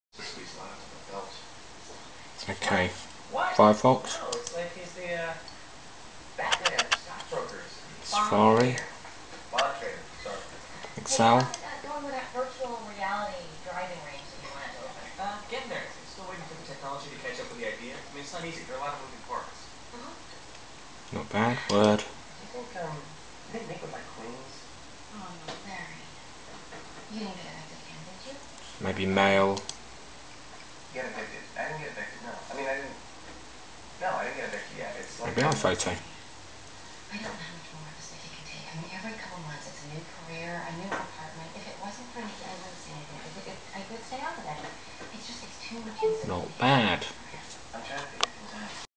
500Gb 7200RPM Applications
Applications being launched on a Macbook Pro with a 7200RPM hard disk.